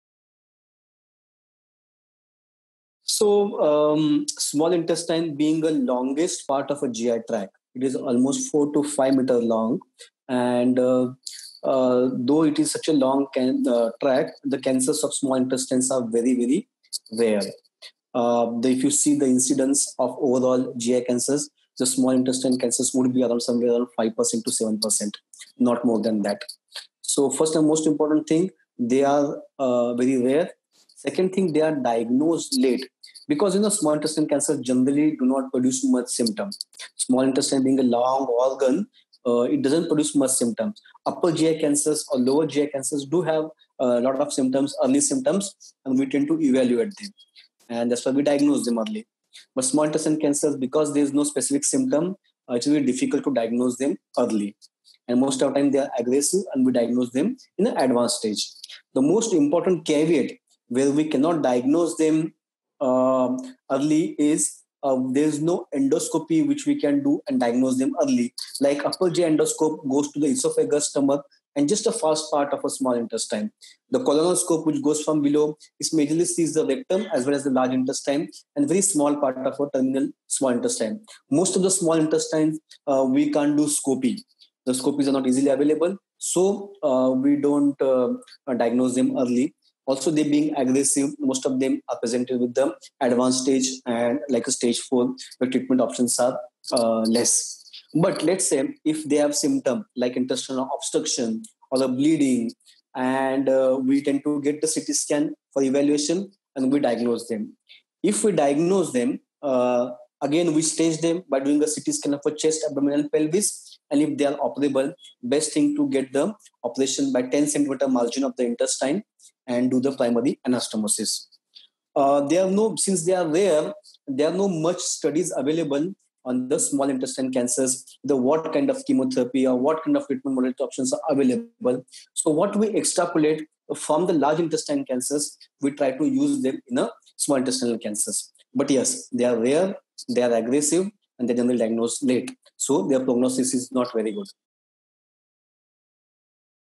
Small-Intestinal Cancer | Dr Imran Shaikh | Cancer Talks
Dr. Imran Shaikh is a board-certified Surgical Oncologist with over ten years of expertise in surgery and Surgical Gastroenterology. He specialises in the treatment of complicated abdominal disorders (GI and HPB surgeries, GI cancers and Liver transplants). Dr. Shaikh took part in our Cancer Talks and gave his thoughts on cancer treatment in India.

Zenonco.io - Making quality integrative oncology cancer care accessible to all.